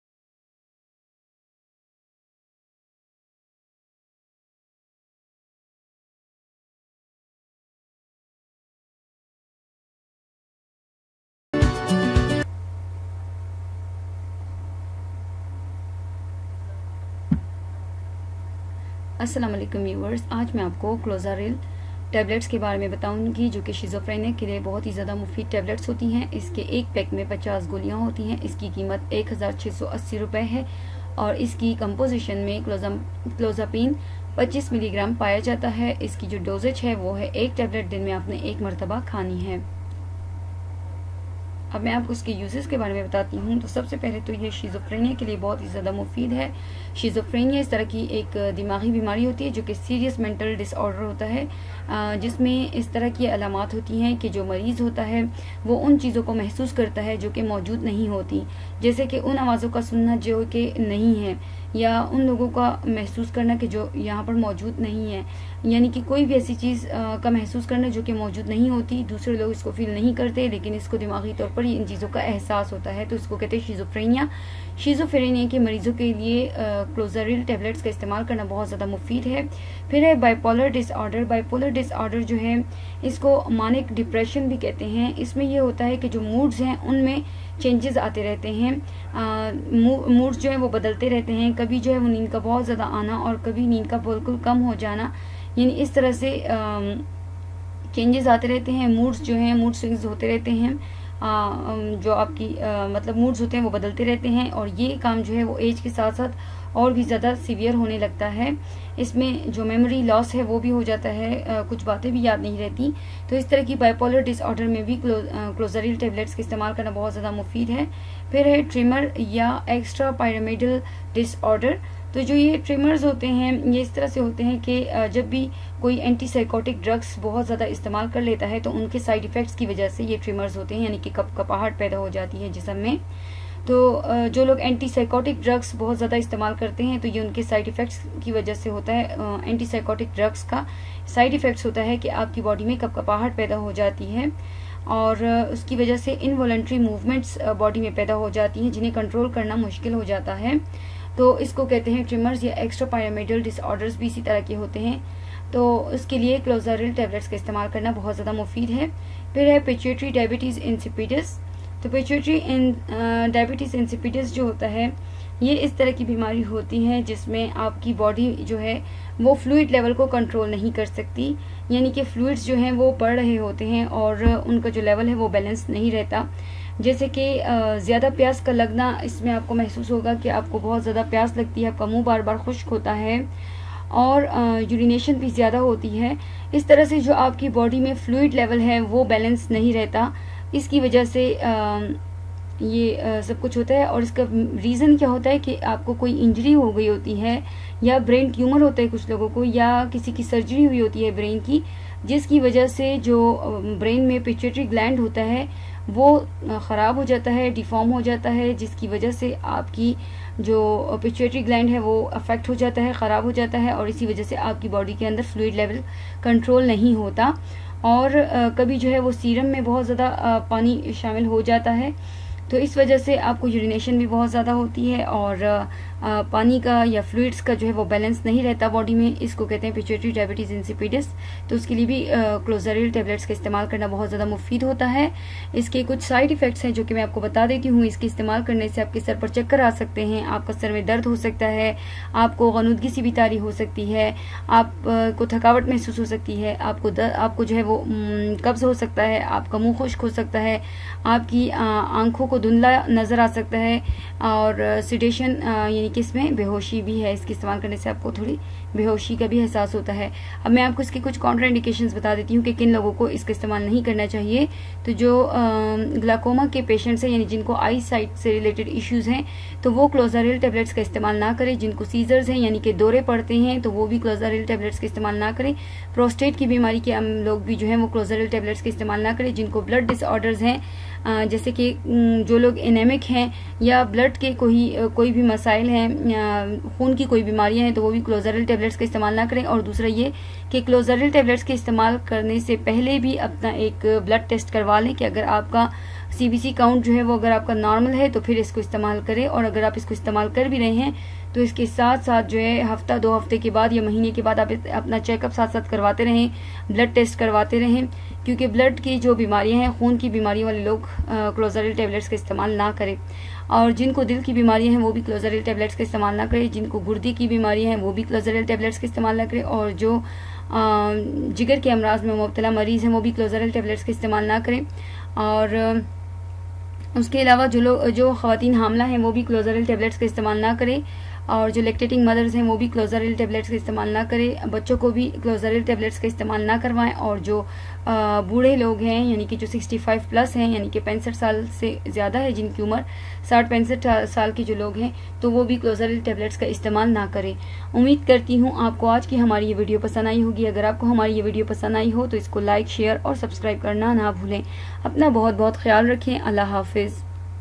Clozaril 25 mg tablets uses in urdu | clozaril tablets for schizophrenia
In this video clozaril tablets are completely discussed.